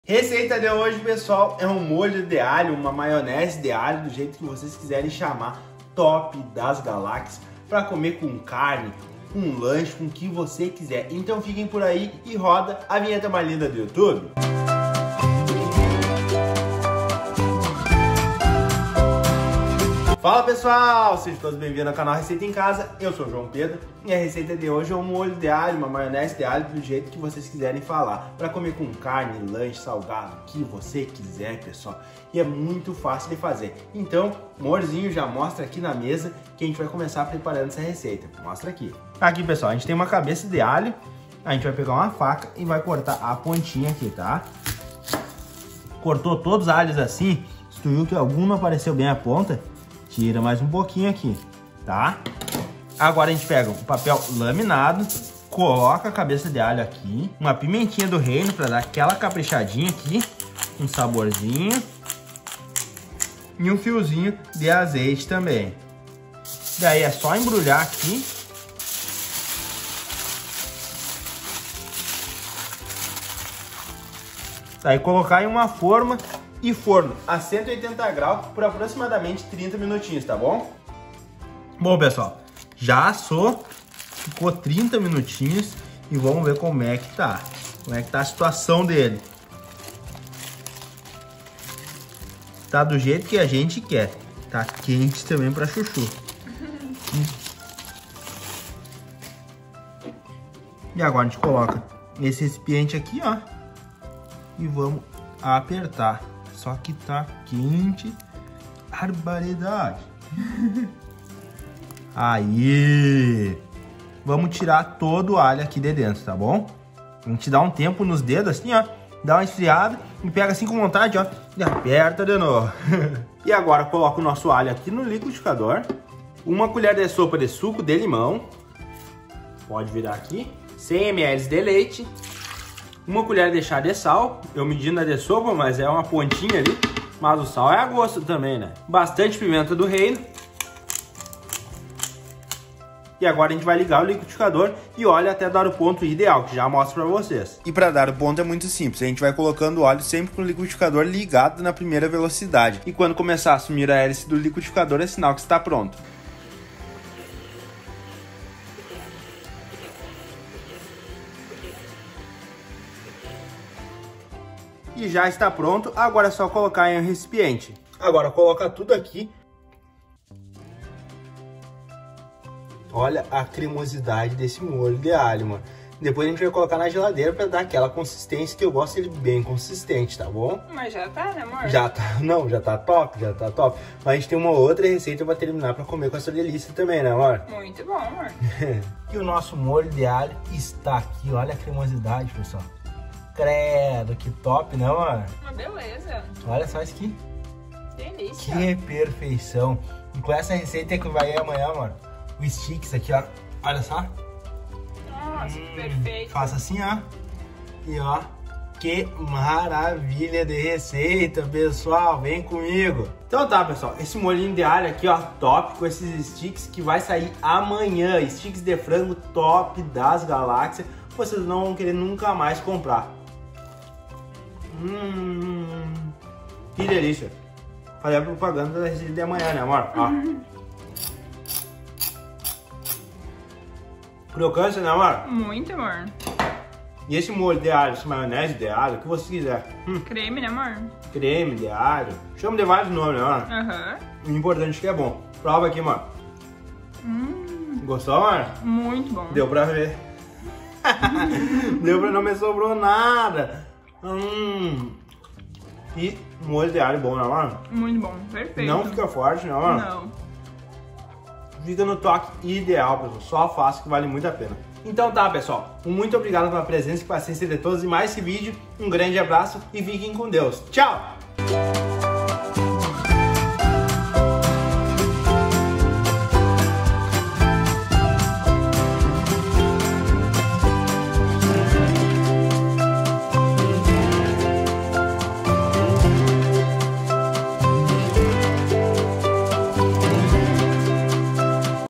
MOLHO DE ALHO | Acompanhamento para carnes e lanches
Essa receita de Molho de alho ficou uma delícia para acompanhar com uma picanha 🥩 mal passada vai ficar top demais 😉, se você gostou da receita aproveita e comenta aqui de qual Cidade e qual Estado você é que vou lhe mandar um grande abraço nos próximos videos.

Pessoal para que o YouTube saiba que você quer receber os meus vídeos, algumas observações
✔Se inscreve

INGREDIENTES DO MOLHO DE ALHO 🧄
1 Cabeça de alho(pimenta do reino e um fio de azeite)
1 Colher de sopa de limão
1 Colher de chá de sal
100ml de Leite
Pimenta do reino
Óleo até dar ponto

OLHE O VÍDEO PARA APRENDER PASSO Á PASSO

Um grande abraço a todos e fiquem com DEUS.